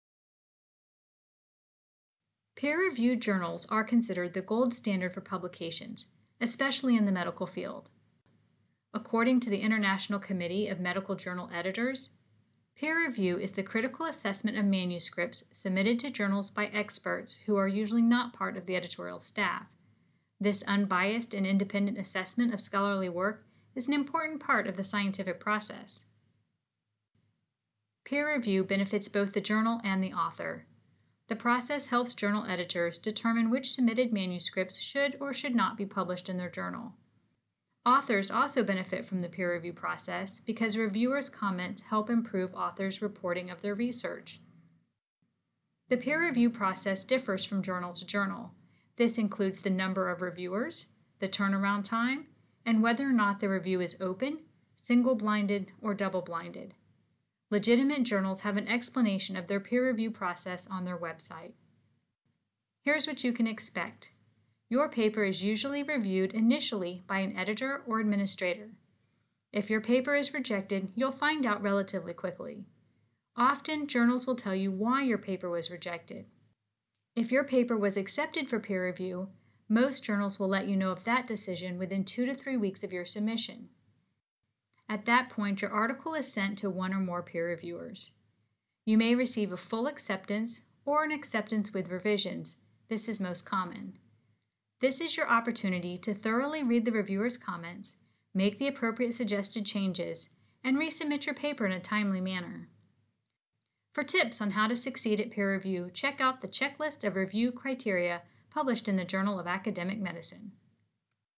Peer Review Process
Learn how the peer review process benefits publishers and authors.